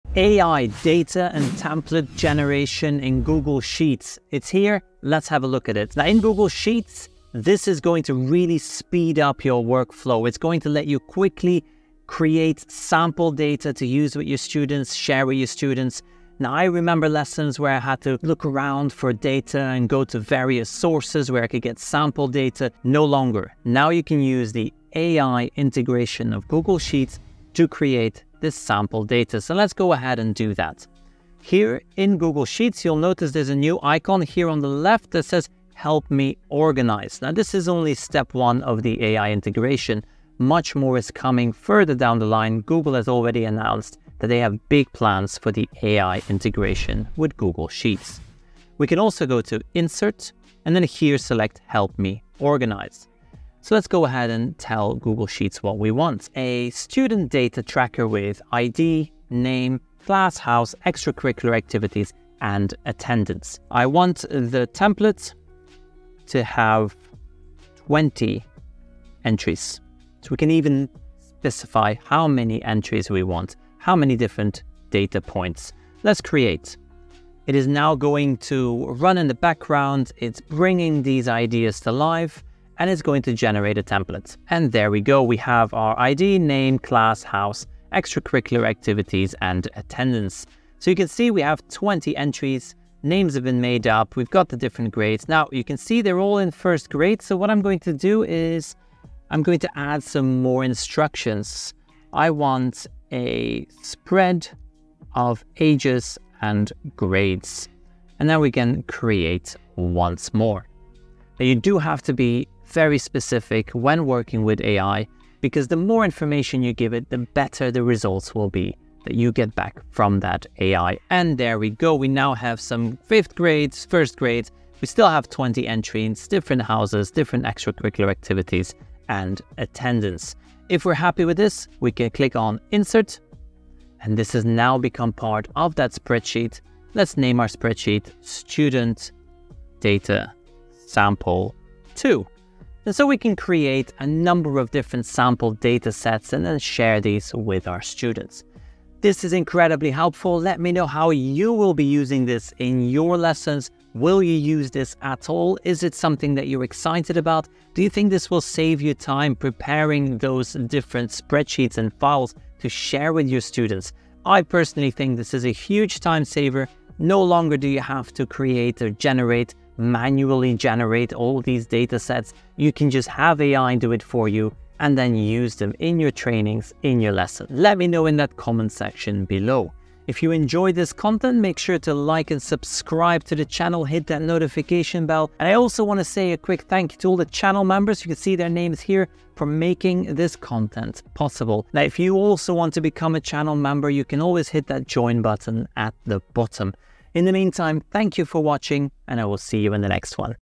AI in Google Sheets is Here | Get Sample Data and MORE!
🟢 AI is here in the form of Duet for Workspace. This is my hands-on demo of how you can use it to create Sample data for lessons and demos.

Thank you all our Channel members! If you'd like to become a member as well follow this link: